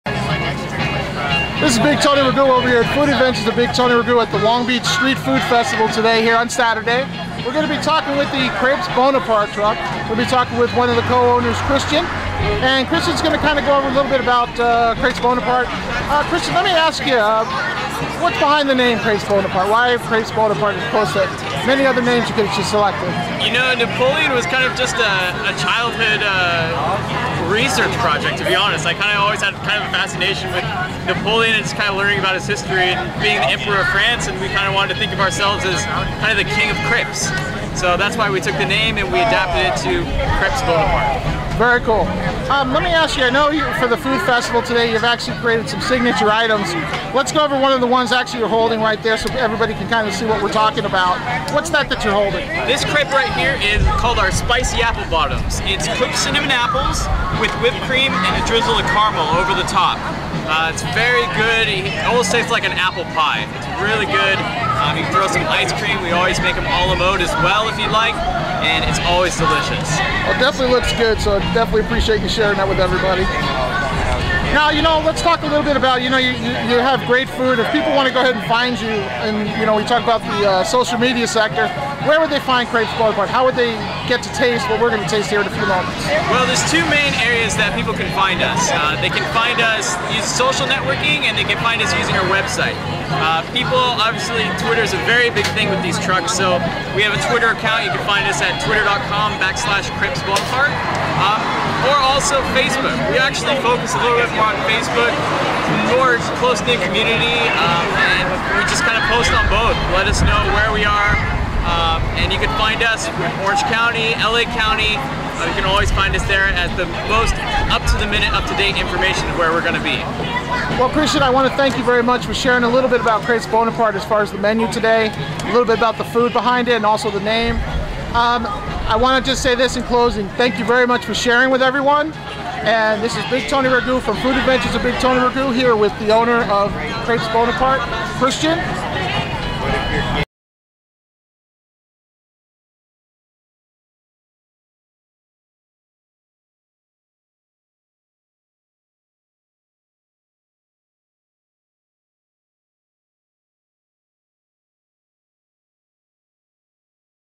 Food Adventures of Big Tony Ragu: Crepes Bonaparte at Long Beach Street Food Fest
When you hear the name "Gaston" you might think of the Walt Disney villian from the animated classic Beauty and The Beast. You would not be far off. The Crepes Bonaparte truck is named "Gaston" and I'm sure its a beast to drive, the beauty is Owners Danielle and Christian Murcia get to make scrumptious crepe creations from the heart of this beast. We sampled a new item called the Spicy Apple Bottoms. Despite the long line, followers praised the Crepes Bonaparte truck team. Enjoy food pic and interview with Christian Murcia. See more information at Food Adventures of Big Tony Ragu